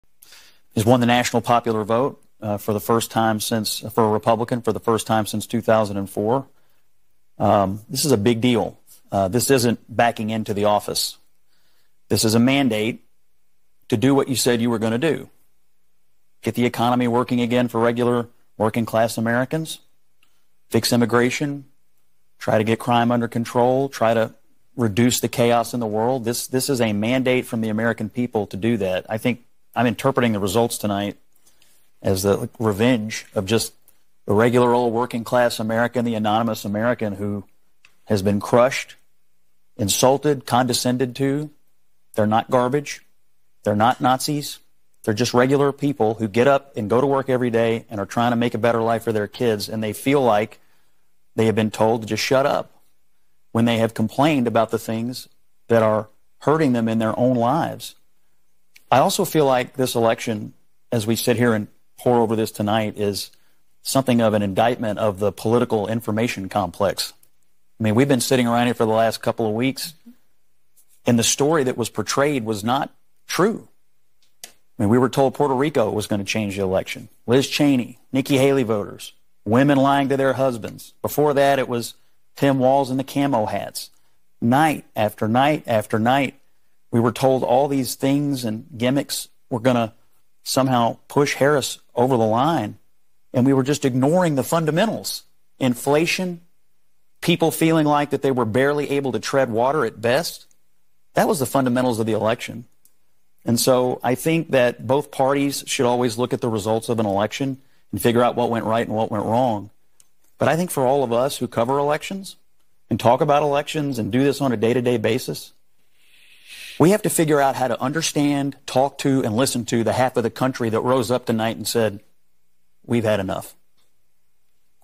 Scott Jennings Response
Regardless of how you voted, you have to admire Scott Jennings for nailing what happened in this nation on November 5th. Note the panel listening intently – no conversation, no interruptions. ￼￼Rare.